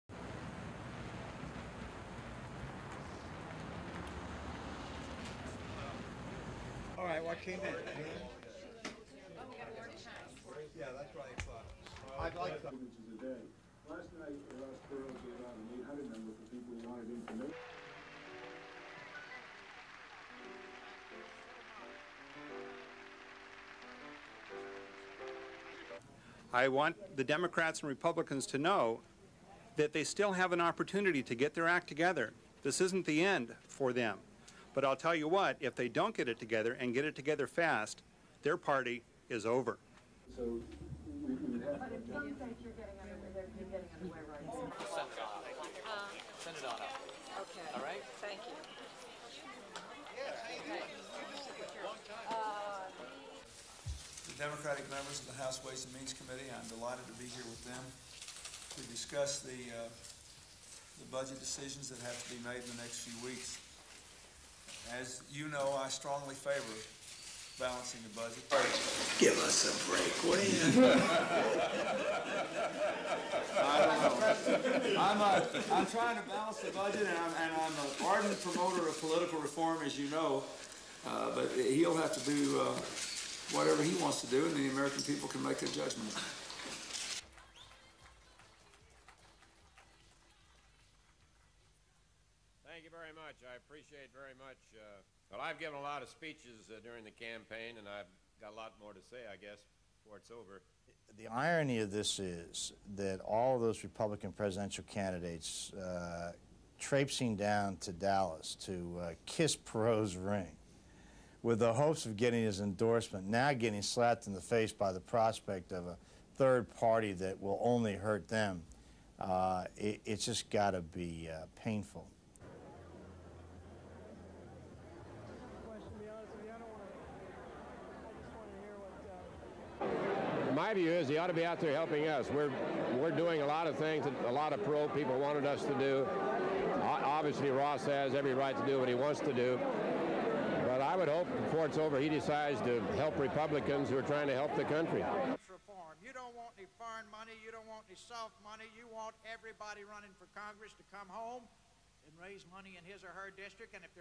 Forming the Reform Party
A news cast about Ross Perot forming of the Reform Party of the United States, and Republican outrage over the forming of the RPUSA.